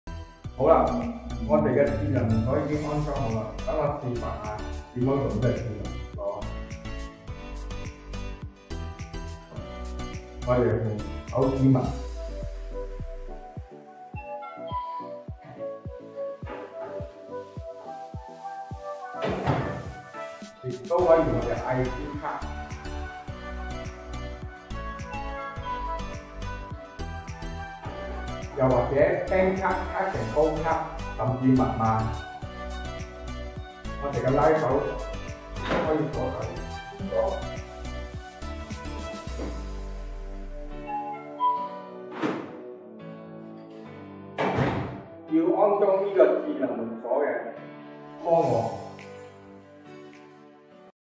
[Thanks to Our Dealer]Abrain Smart Door Lock Demo on Security Door - AHD Malaysia
[Thanks to Our Dealer]
Mr Harry @ Craftmaster @ Ipoh
Demo On Security Door
How its Operate

怡保广东话版本
智能门锁开门的方法‼
装上“智能门锁”后, "天地钩"仲可唔可以锁？
依家就示范 俾你看！

Find us if you need to install Smart Door Lock on your Security Door.

Want to know more or need our consultation?
Find us now!

Facebook : AHD SMART HOME @ SMART ALARM